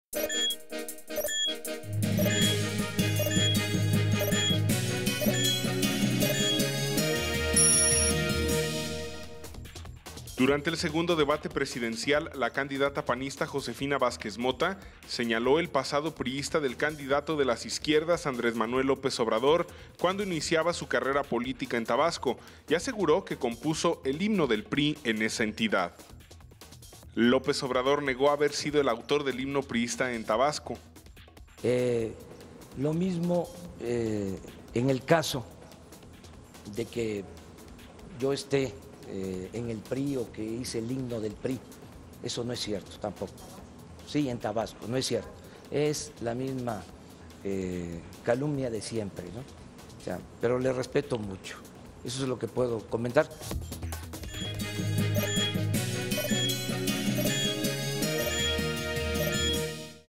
Corte 44 - Josefina Vázquez señaló a AMLO por su pasado priista - Junio 11 de 2012
Durante el segundo debate presidencial la candidata panista, Josefina Vázquez Mota, señaló el pasado priista del candidato de las izquierdas Andrés Manuel López Obrador, cuando iniciaba su carrera política en Tabasco,  y aseguró que compuso el himno del PRI en esa entidad.

López Obrador negó haber sido el autor del himno priista en Tabasco.